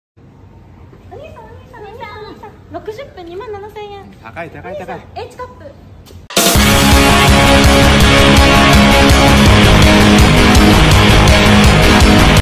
touhou sanae fans be like
alternative title: 27000 Yen for 60 minutes (Sanae's breast edition). i reuploaded it because of freaking hate shorts.

POV: you are walking around after work. suddenly, most sane gal in touhou will offer you 27000 yen for 60 minutes to make fun with :sustroll:

this is last video of september before october lol

sauce: 412174